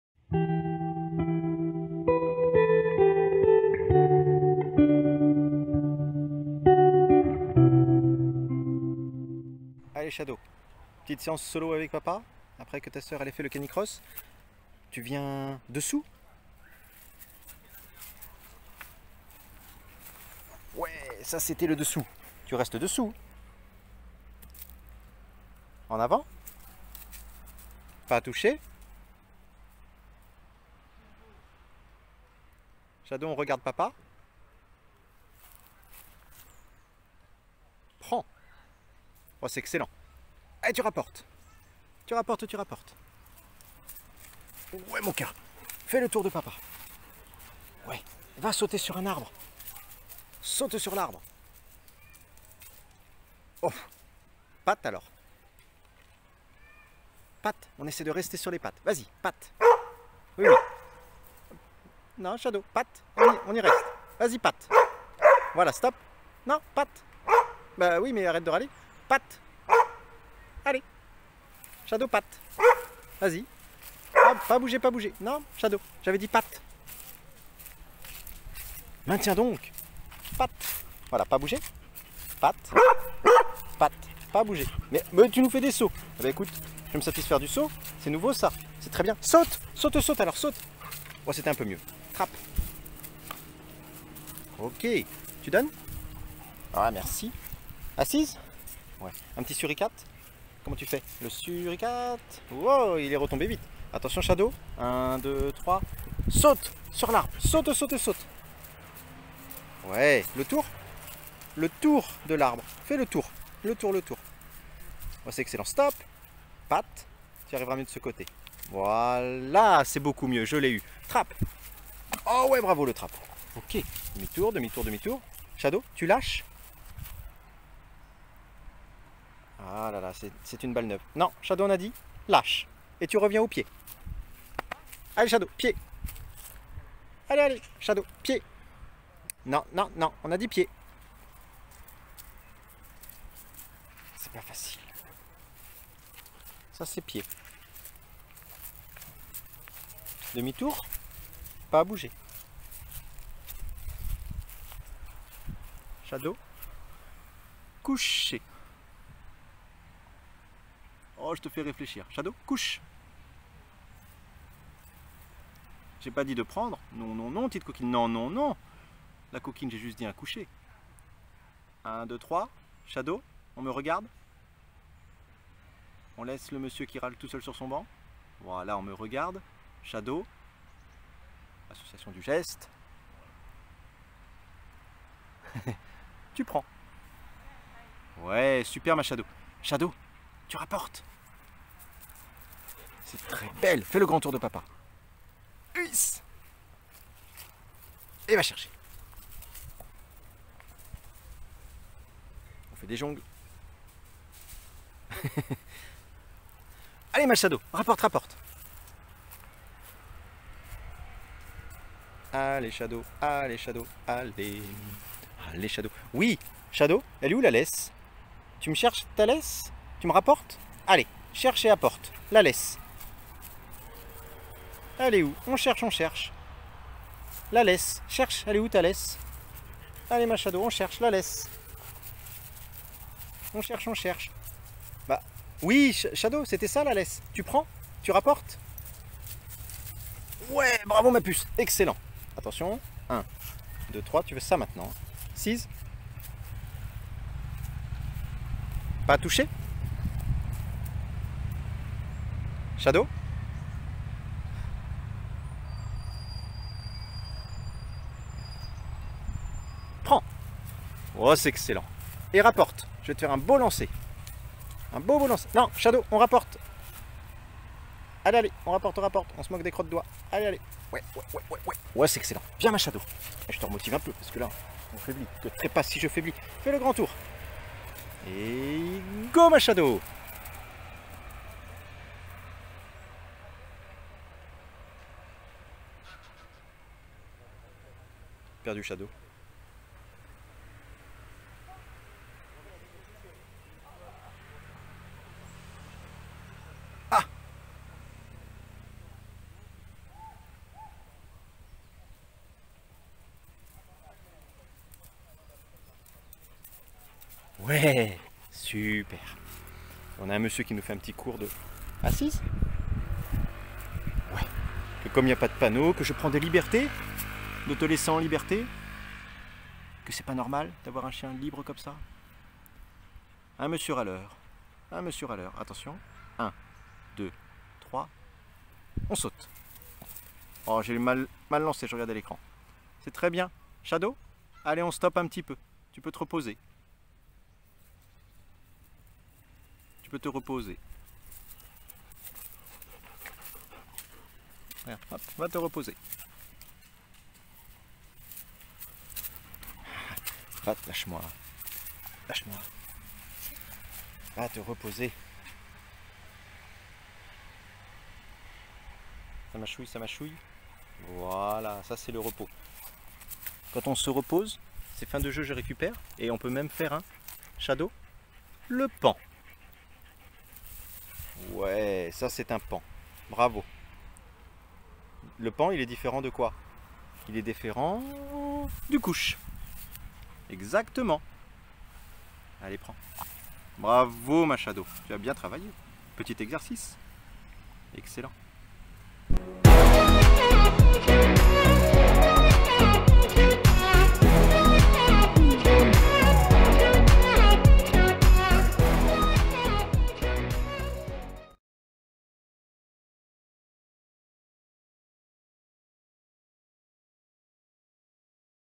Exercices, Jeu et Obéissance avec Shadow
Dans cette vidéo, nous vous proposons une séquence complète de jeu avec Shadow durant laquelle nous répétons des acquis, continuons l'apprentissage de certains ordres et créons de nouveaux enchaînements.
Vous verrez nos façons de réussir ensemble, nos échecs et corrections pour parvenir à nos objectifs.
Et en fond, il s'agit surtout de passer un moment de partage et de motivation avec son chien.